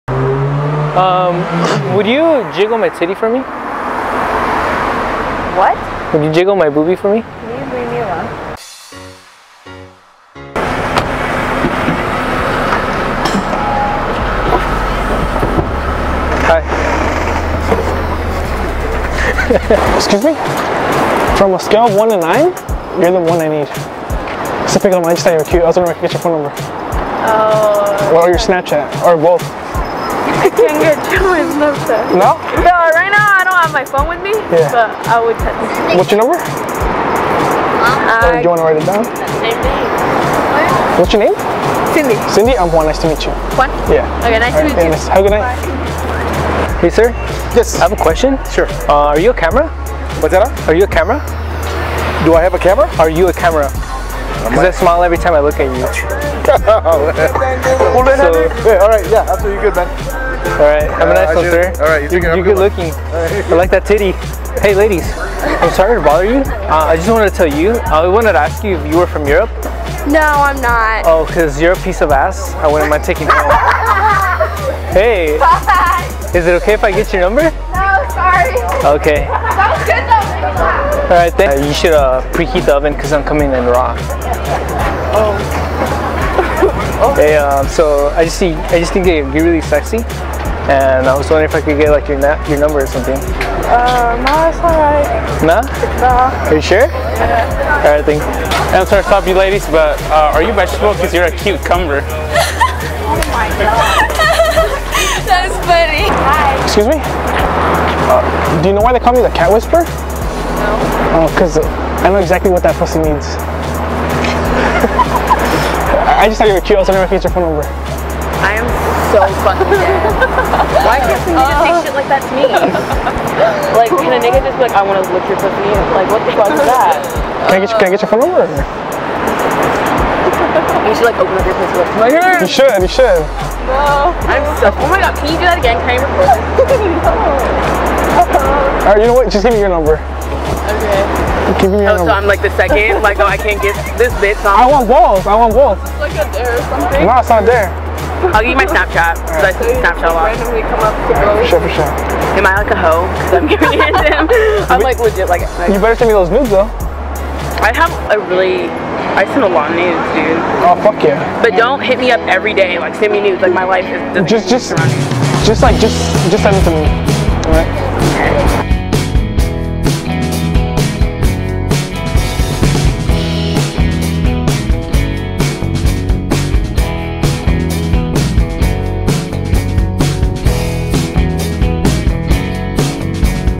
Do you know why they call me the cat whisperer?
We use your youtube comments and use them on random people. If you guys enjoyed the video make sure you leave more comments for a part 2.Social Media Links: